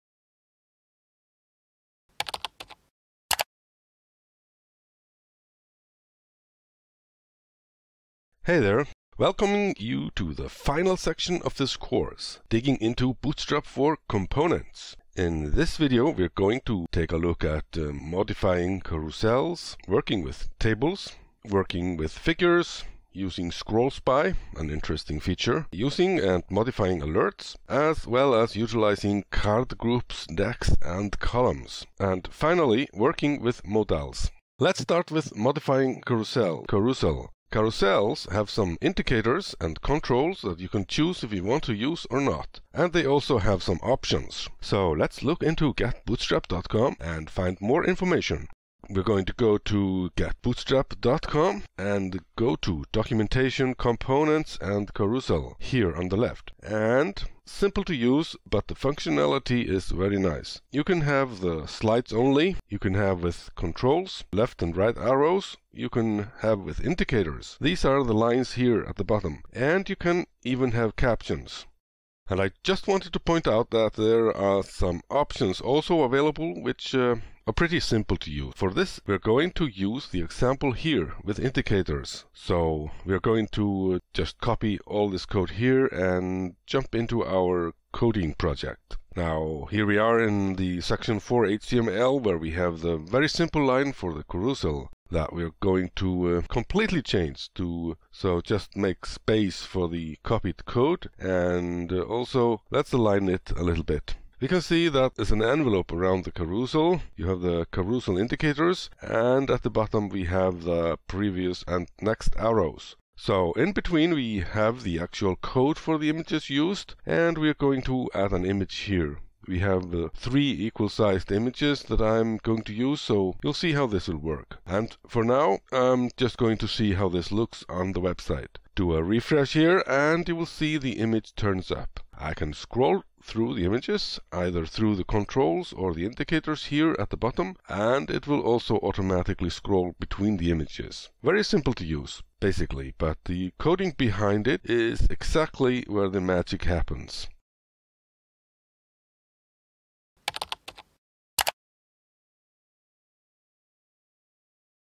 Bootstrap 4 Recipes : Modifying Carousels | packtpub.com
In this video, we discover carousels and understand how to modify them.
• Utilize controls and indicators
• Understand options